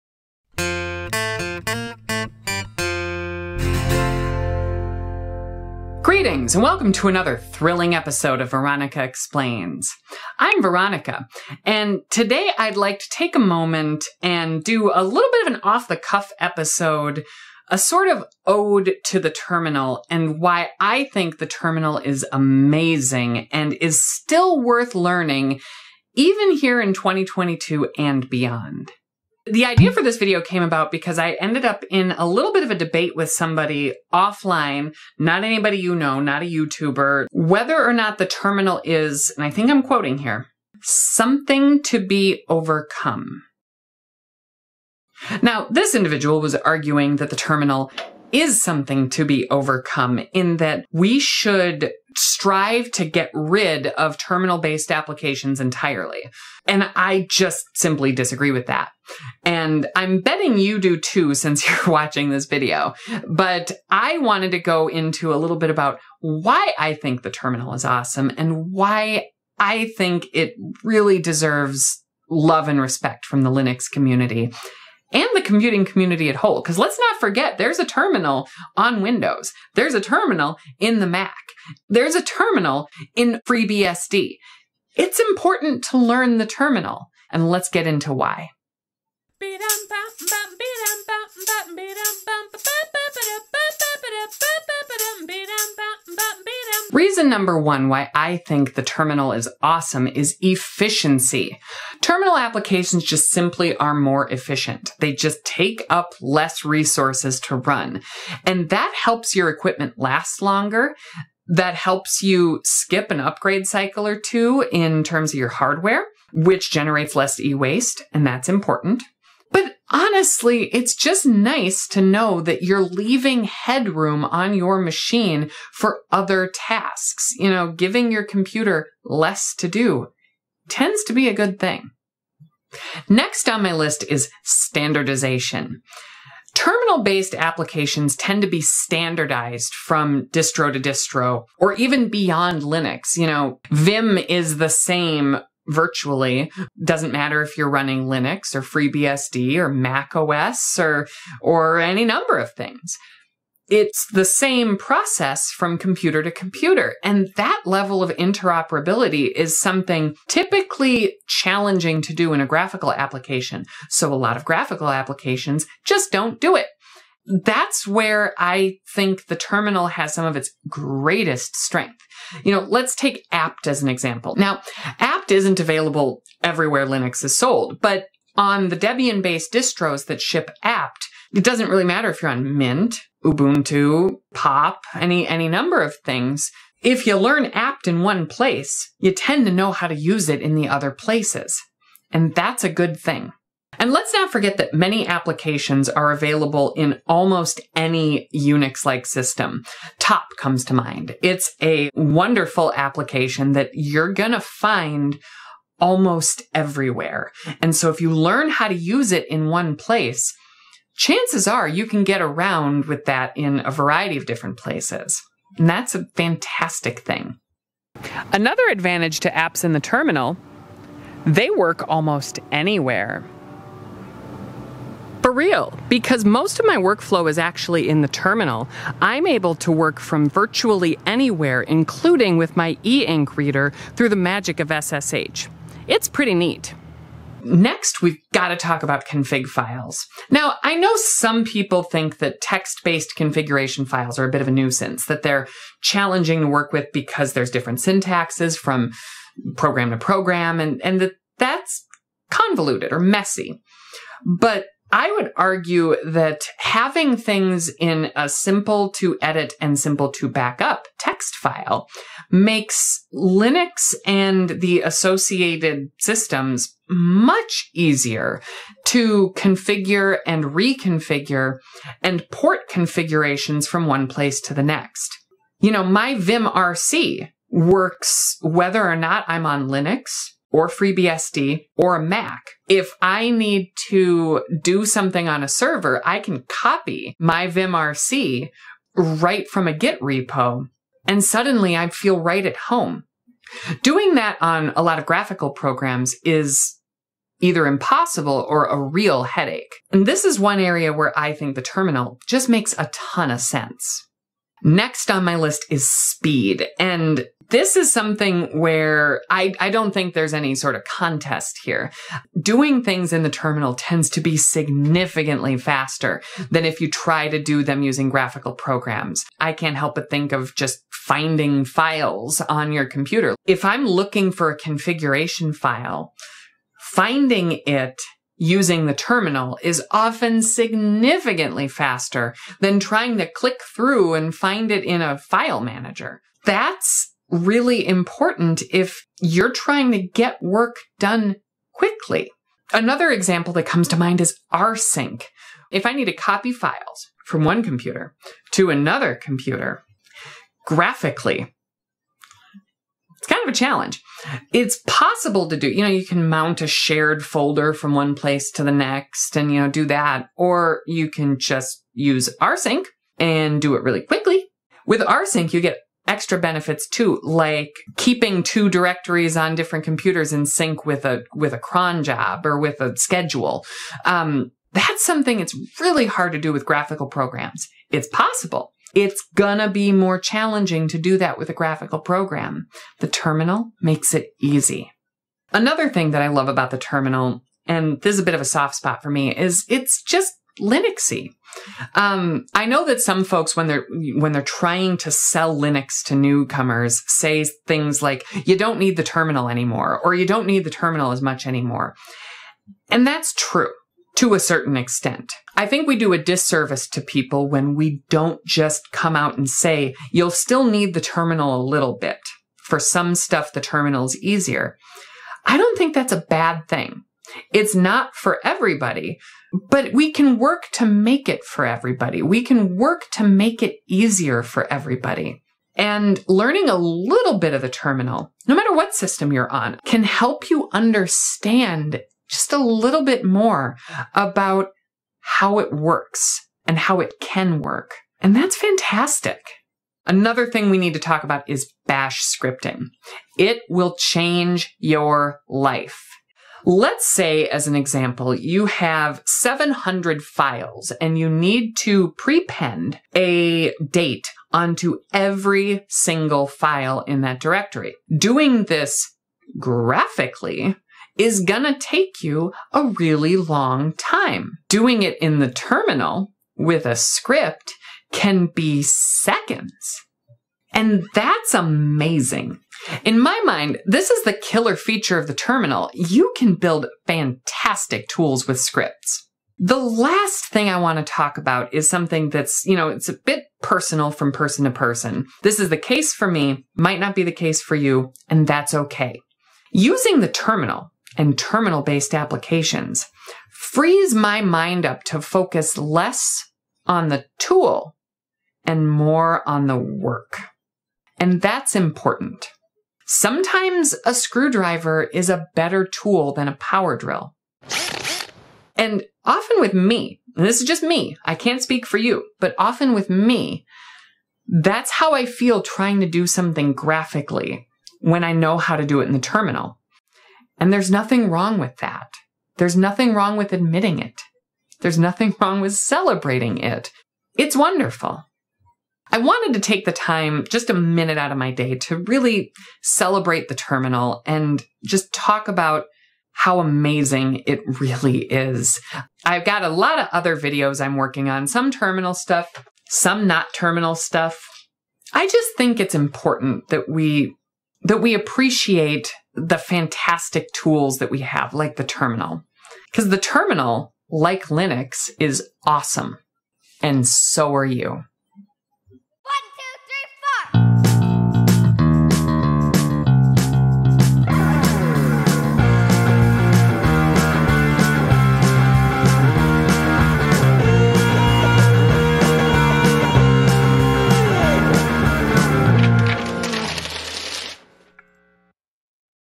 Is the terminal still worth learning?
YES. You should still learn the terminal!

Whether you're just a casual Linux user or planning a career in tech, learning the terminal is a great way to improve efficiency, gain insight into your software and hardware, and focus less on the tools themselves.

In this video, I casually explain why I think the terminal is still worth learning. Unlike most of my videos, this one was very free-wheeling- enjoy!

Proceeds benefit the channel!

00:00 Intro
01:36 Terminal tools are more efficient
02:12 Terminal tools are standardized across systems
03:57 Config files are better than playing with menus
05:04 Terminal apps are faster!
06:25 The terminal feels great as a Linux user
07:20 Bash scripting will save your life
07:59 Focus on the tool, not the work
08:49 Outro